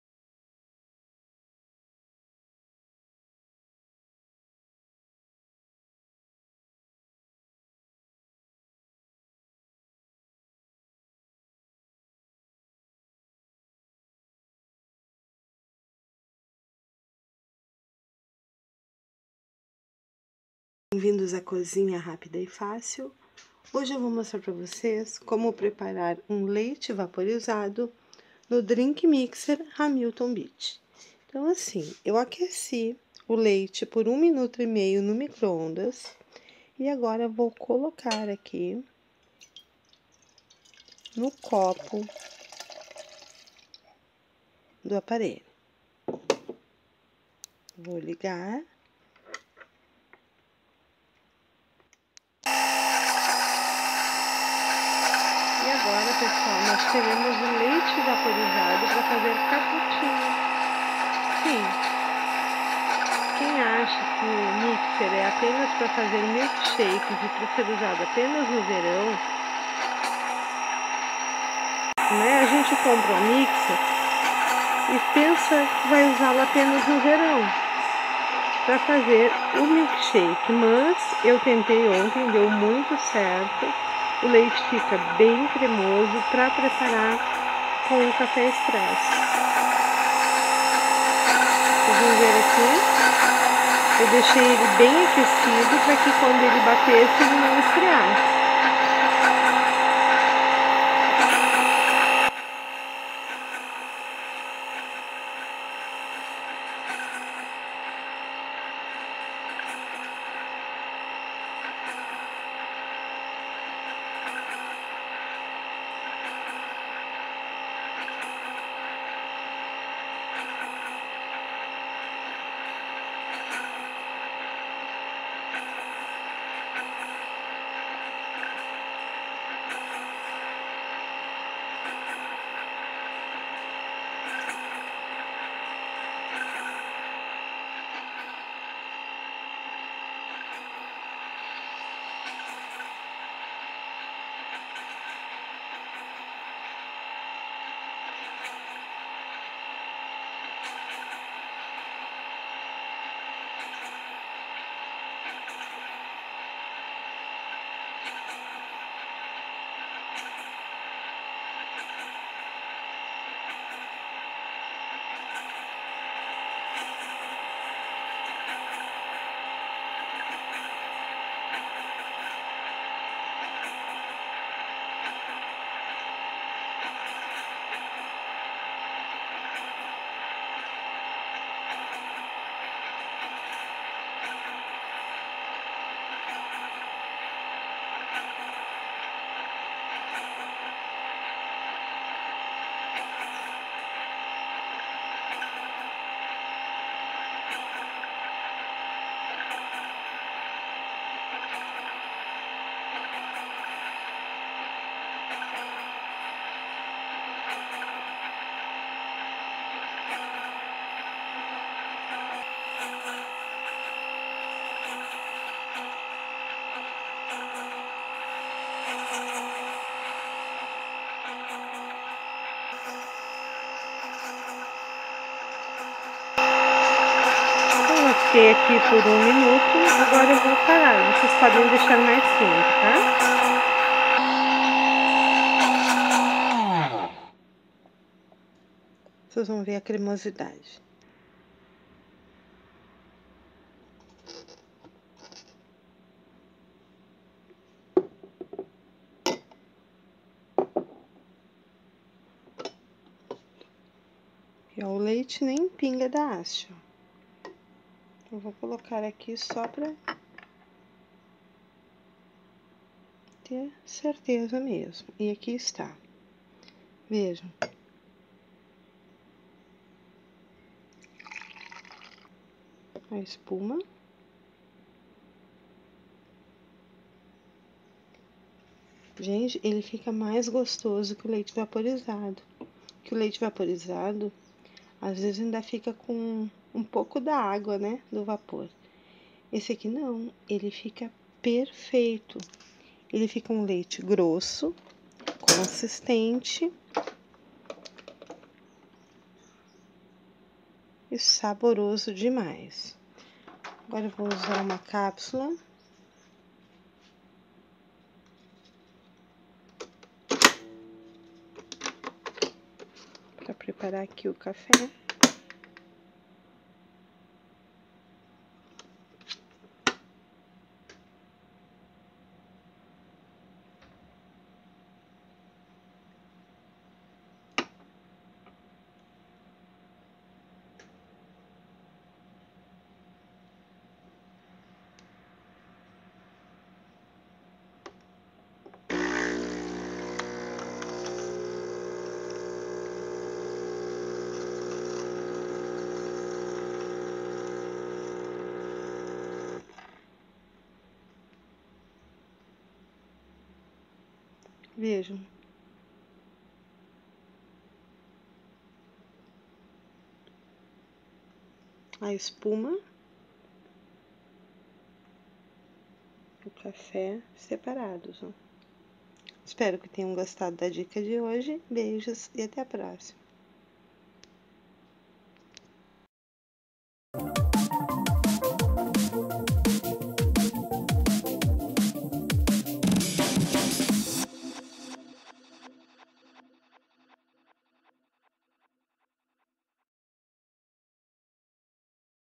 Como fazer Leite Cremoso com o Drinkmixer Hamilton Beach!!
Que ele é lindo eu já sabia,mas que era perfeito,,,isso fiquei sabendo hoje depois que utilizei!

Amo eletroportáteis e à cada nova aquisição mais me apaixono por eles.

Usei ele para fazer um Leite cremoso muitoooo delicioso,veja no vídeo!